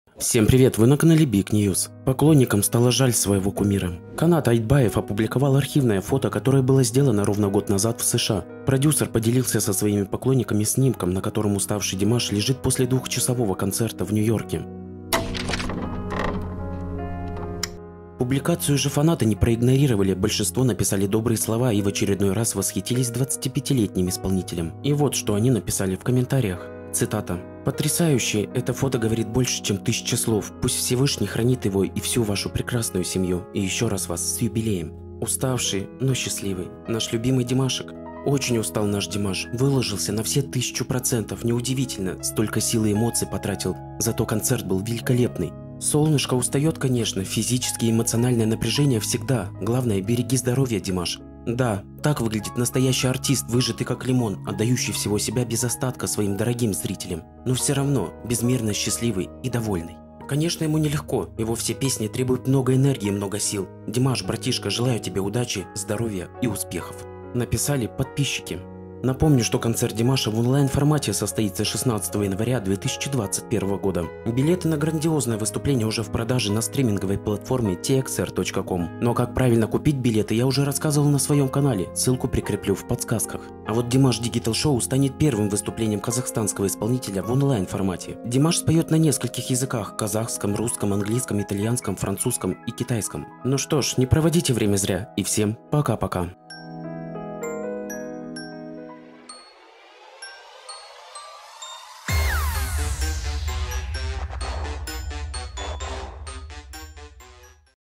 Отец Димаша напомнил, что происходит с сыном после...
Канат Айтбаев (отец Димаша) опубликовал архивное фото, годовой давности из США. Где показал, что происходит с сыном после...